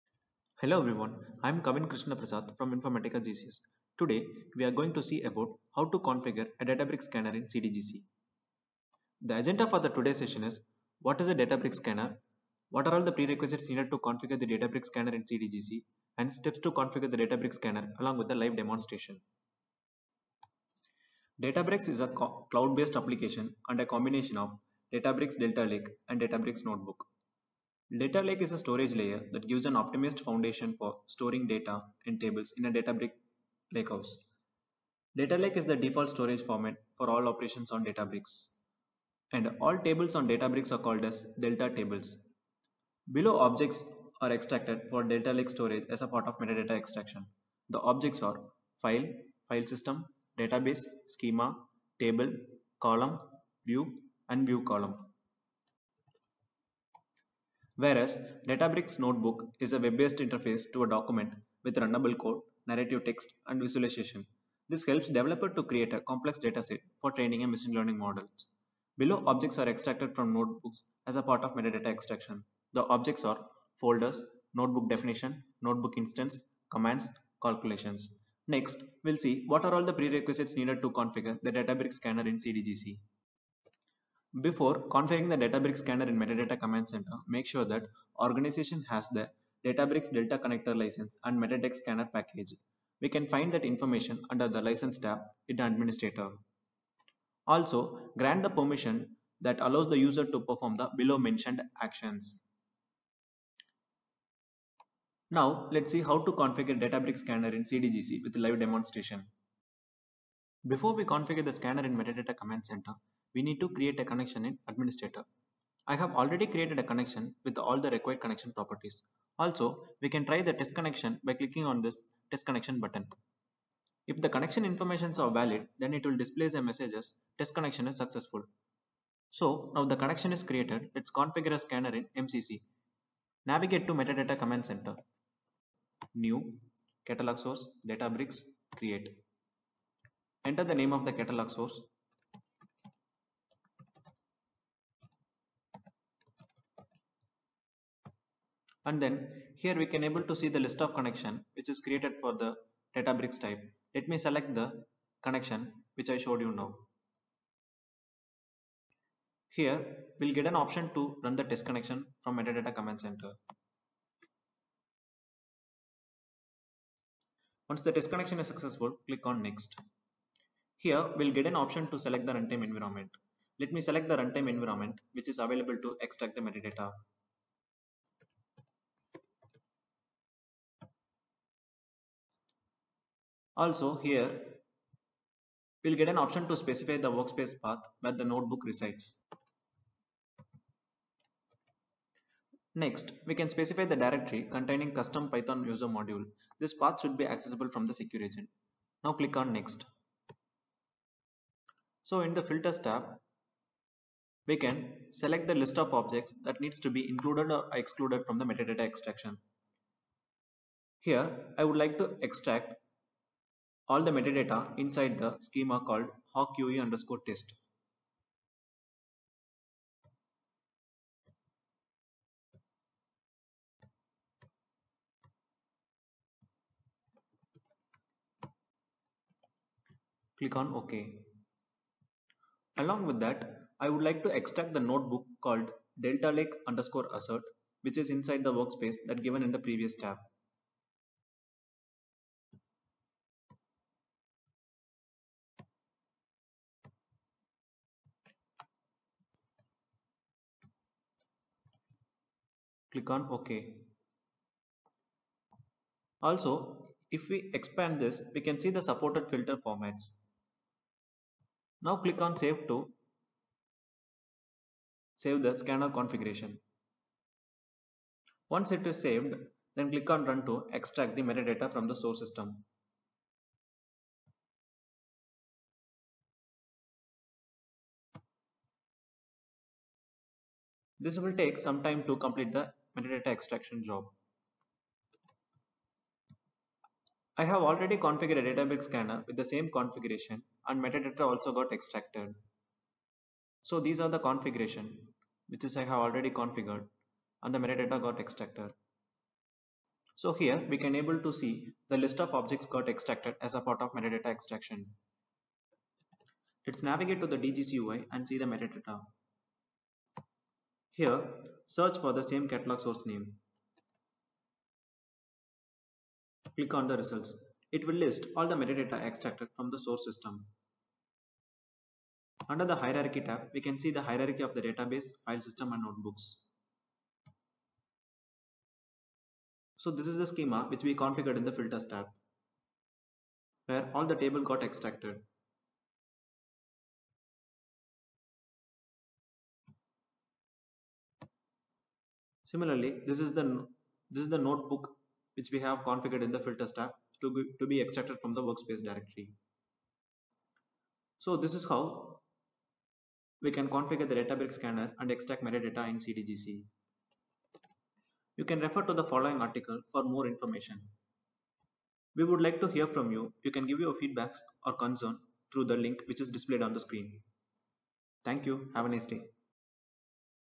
302. How to Configure a Databricks Scanner in CDGC
This video explains how to configure a Databricks Scanner in CDGC.